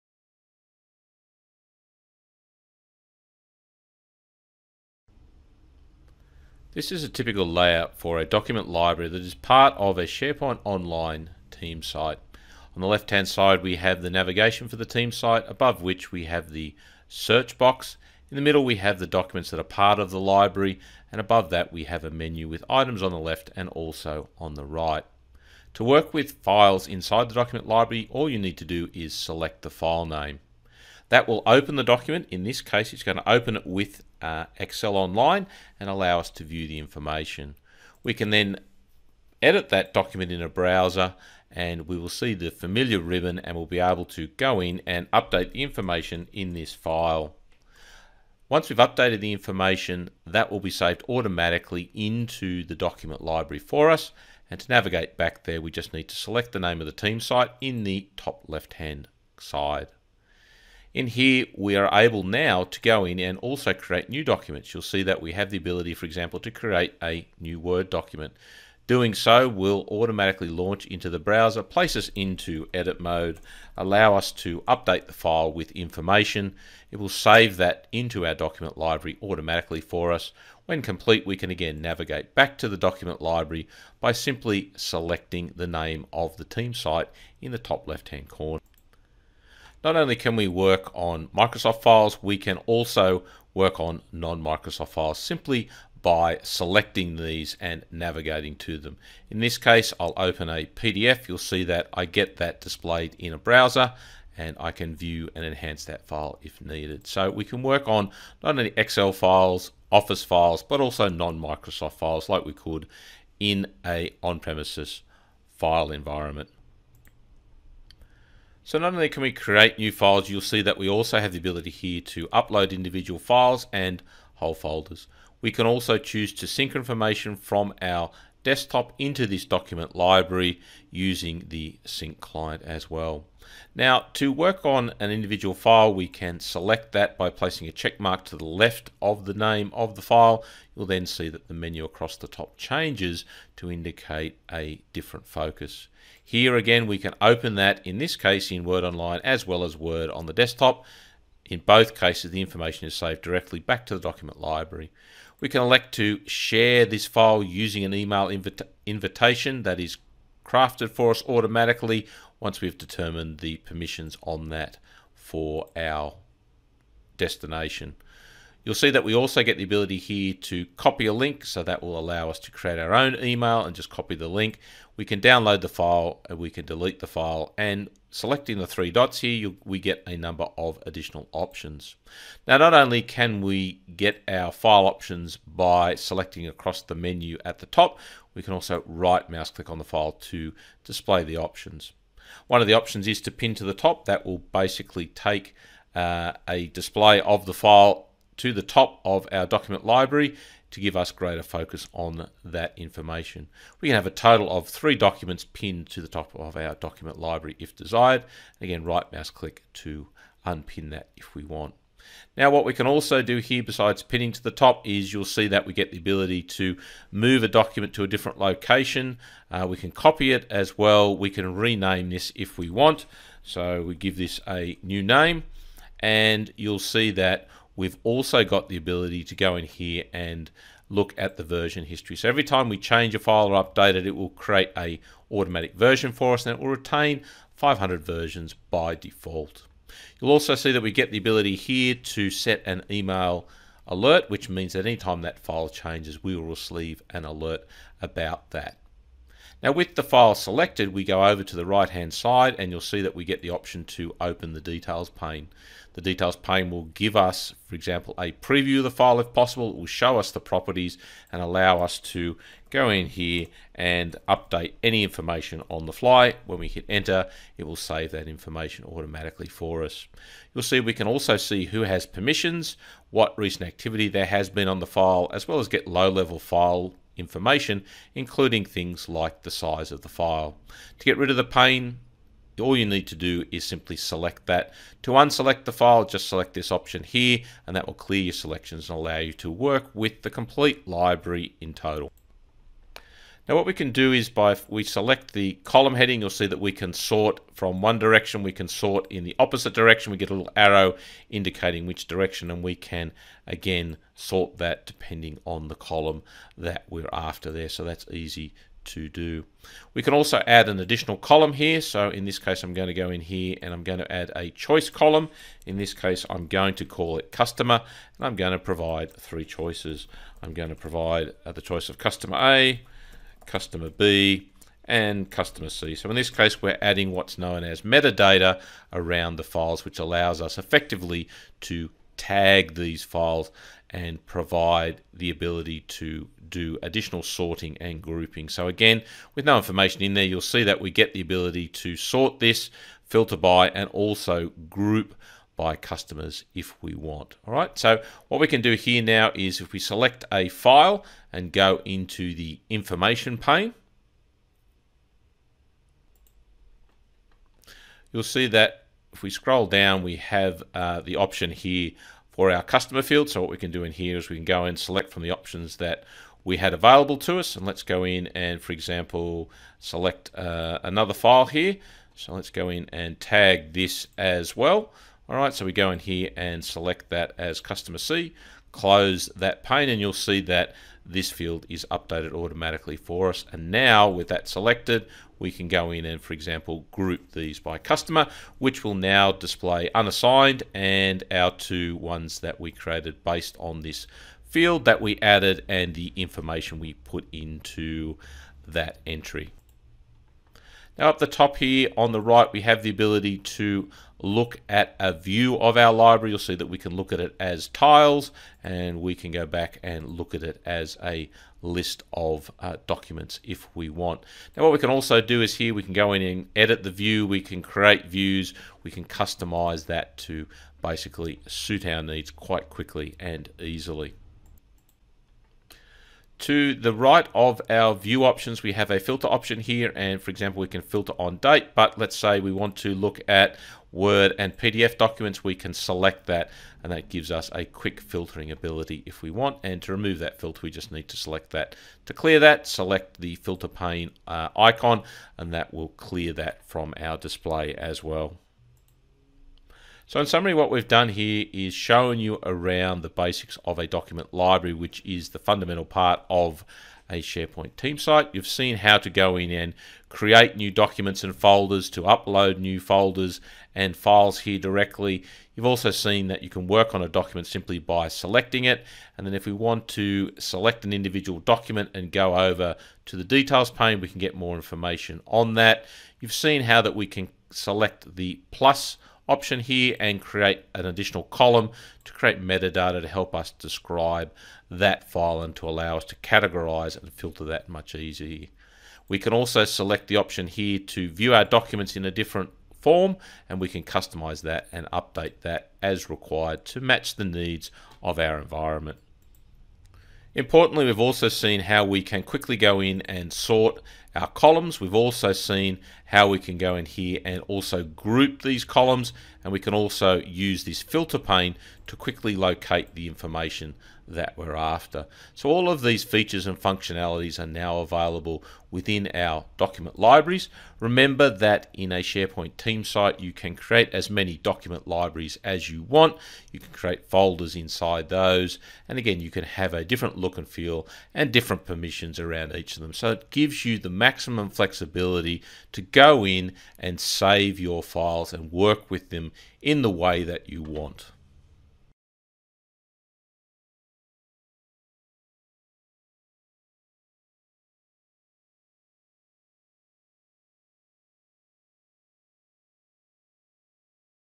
Basics of working with Document Libraries
This video show you how to get started with the updated interface for SharePoint Online Document Libraries. You'll see how to create file in the browser, view the properties, sort and filter. You'll see how to create an additional 'metadata' column to help categorise your information. If you need to get up to speed quickly with the updated look and feel of SharePoint Online Document Libraries, start here. August 2017 update for latest changes to Office 365 interface.